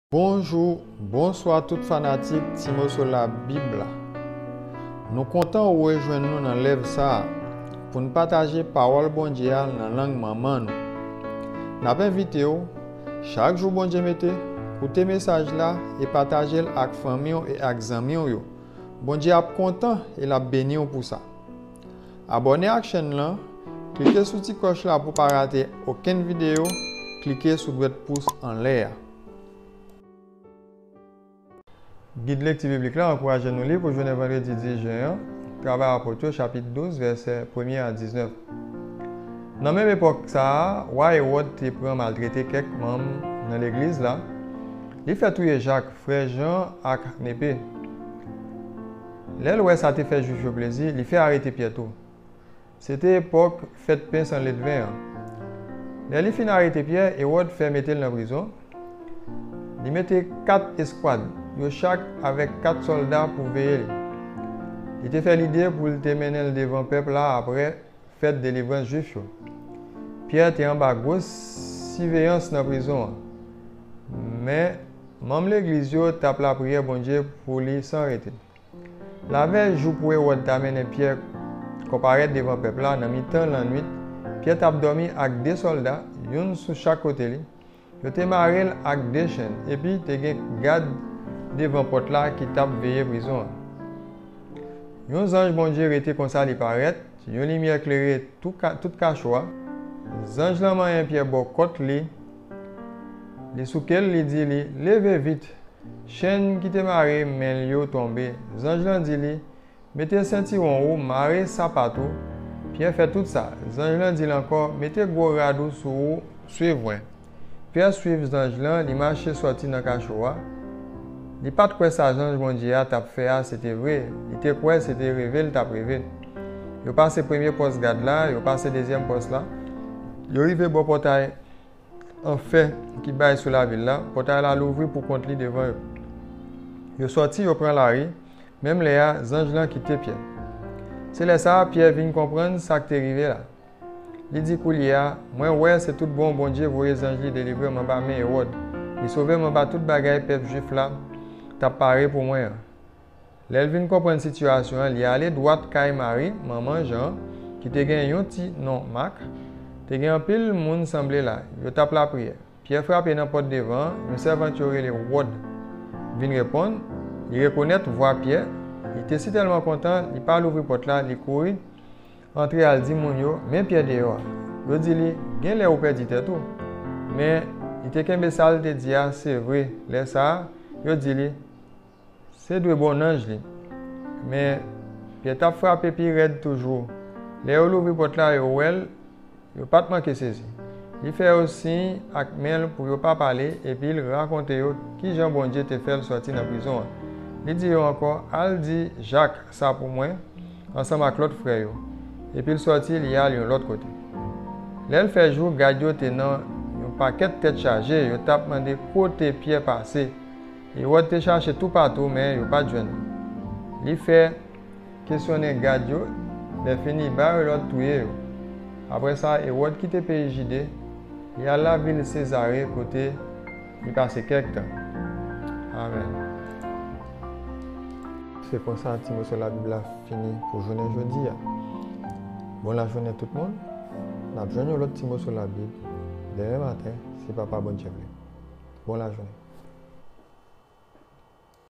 VANDREDI 10 JEN TRAVAY APOT 12 V 1 A 19
GID LEKTI BIBLIK LA ANKOURAJE NOU LI E MEDITE SOU PAWOL SA YO POU JODIA VANDREDI 10 JEN
KE NAP JWENN NAN TRAVAY APOT 12 V 1 A 19